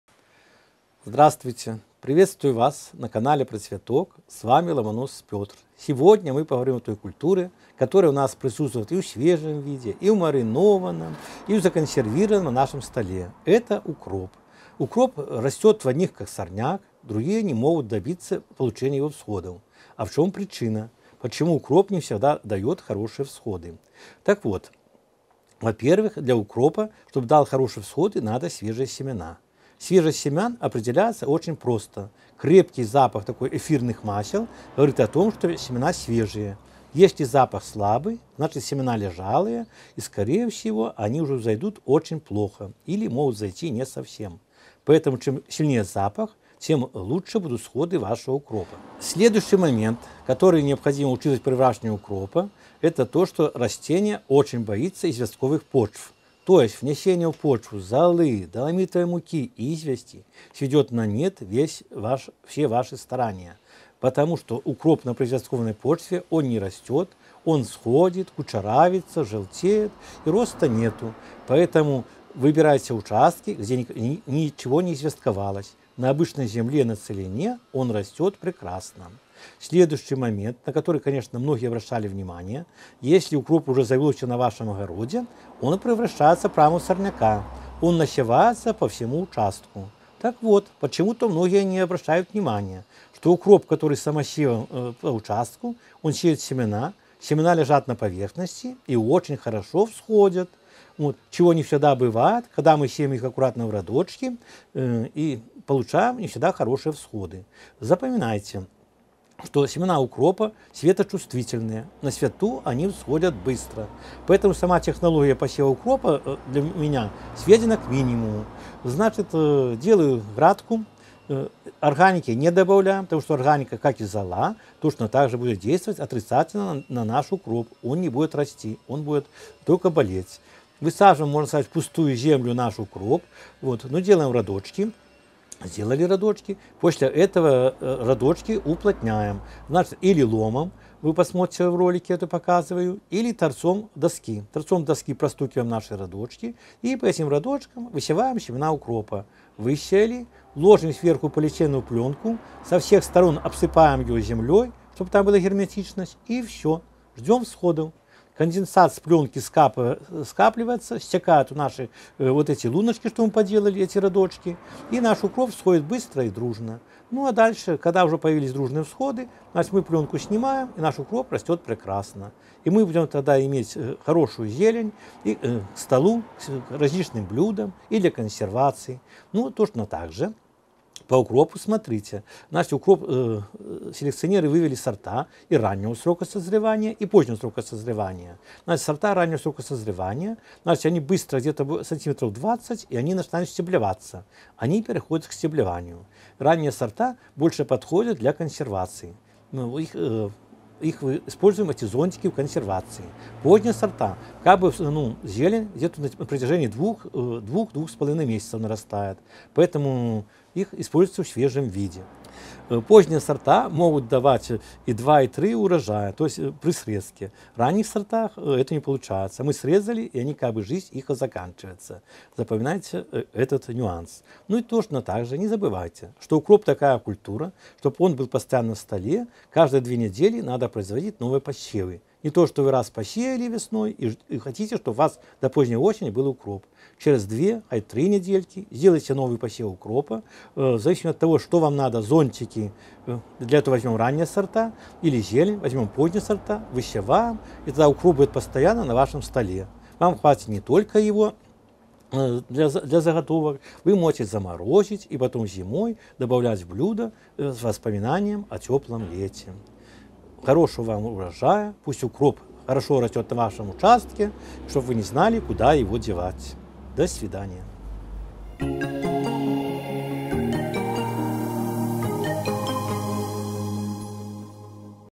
УКРОП ВЗОЙДЕТ ЩЕТКОЙ! Посейте так укроп, и быстрые всходы гарантированы!
Как вырастить пышный ароматный укроп? Почему семена укропа не всегда дают хорошие всходы? Что не любит эта зеленная культура и какие нюансы еще важно учесть при посеве укропа, рассказывает опытный агроном Петр Ломонос.

00:00 Вступление
00:24 Как определить свежесть семян укропа
00:48 Укроп не любит щелочную почву
01:19 Семена укропа лучше всходят на свету
01:54 Как посеять укроп – авторская технология
02:59 Особенности сортов укропа разных сроков созревания
03:44 Сеем укроп несколько раз за сезон